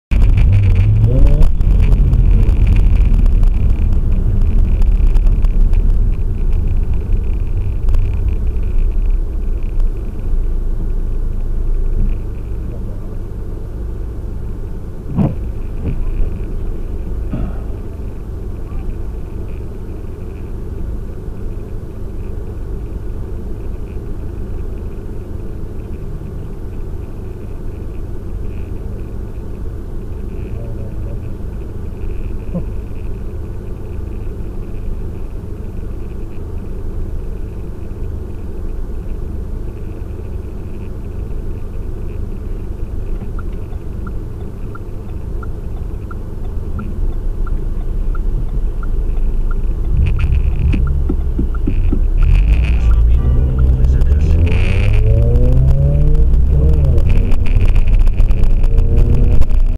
Ce conducteur aurait du rester dans sa voiture
Dans une ville en Russie, le conducteur d'une voiture grise va sortir pour régler ses comptes avec le conducteur d'une voiture rouge. Mais il n'aura pas le temps de dire ou faire quoi que ce soit, que le conducteur de la voiture rouge va le mettre à terre en lui donnant un coup de poing. Il aurait mieux fait de rester dans sa voiture